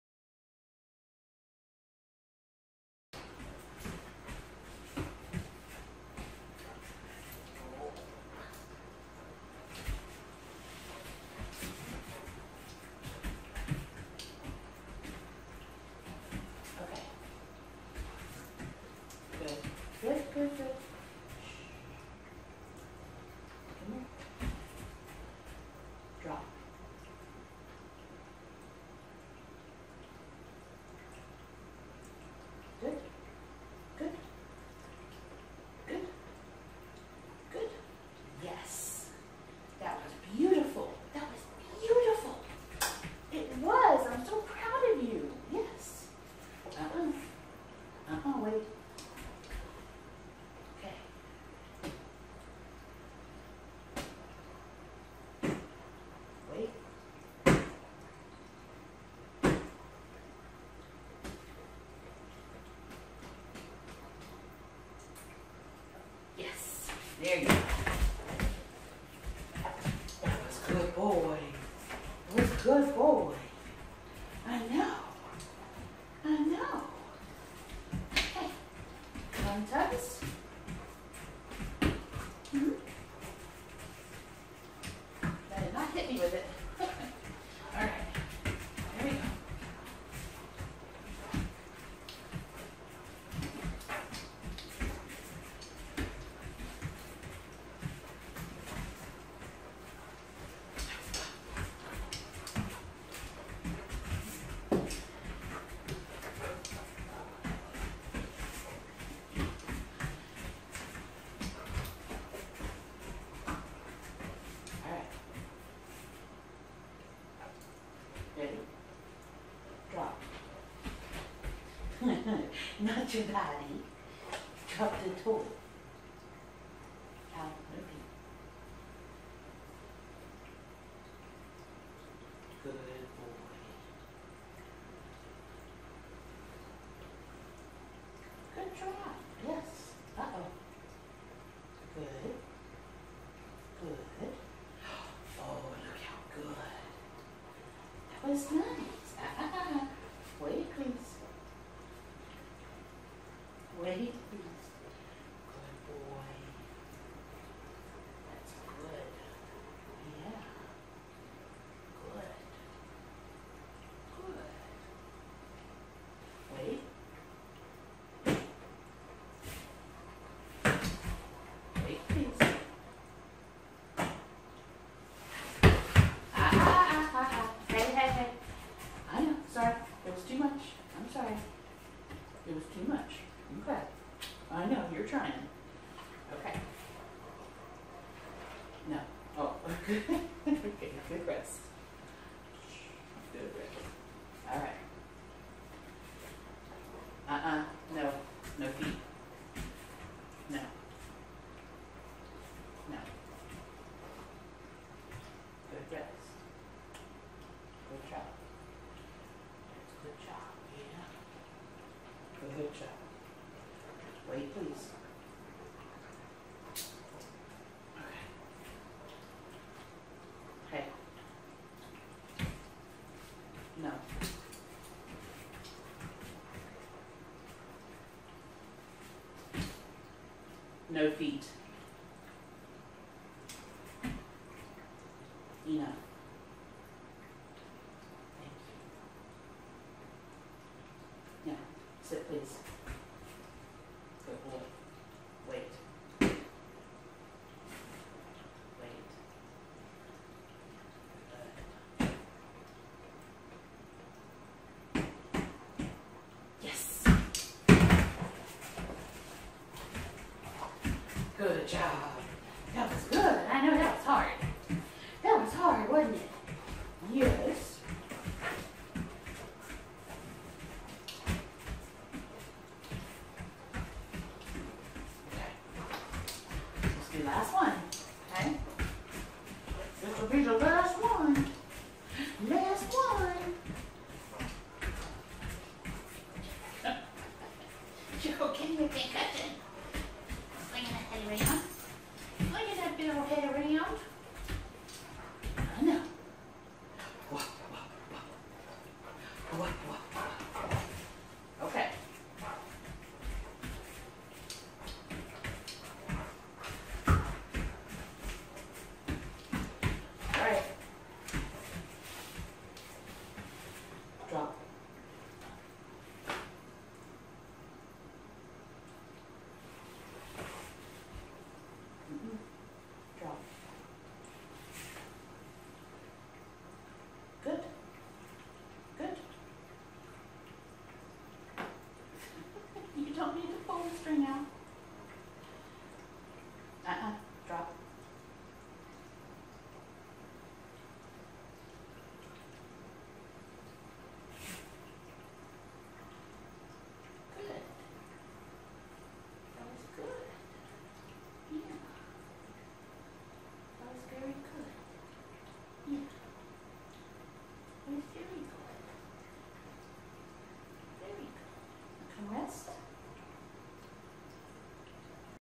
Jasper Impulse Control Toy
Jasper has a great toy drive - but he gets overly excited especially when frustrated - and redirects onto whatever is nearby - using his mouth, his feet or his body to control the situation.  We are working on him learning alternative ways to handle his frustration calmly.